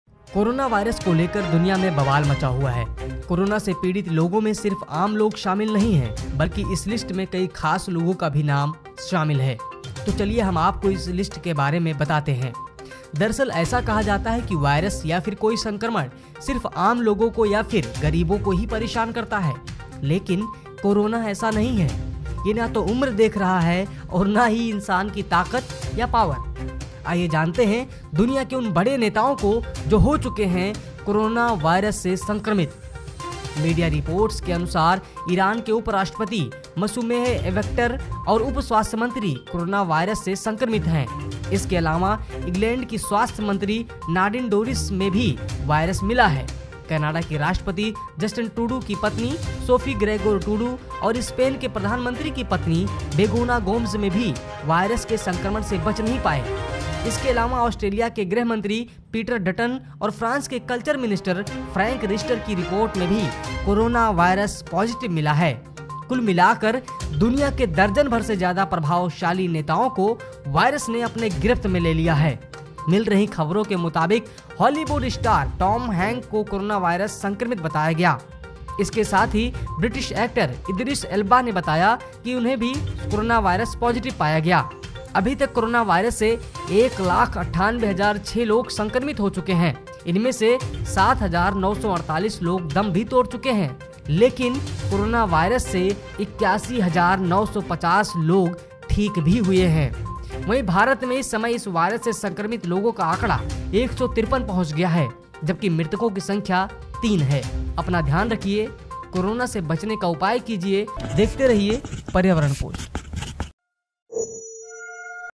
दुनिया के बड़े नेता भी CORONAVIRUS से नही बच पाये, जानिए कौन कौन हैं संक्रमित!
Team : Delhi
Research & Script:
Video Editor: kuldeep
Voice Over :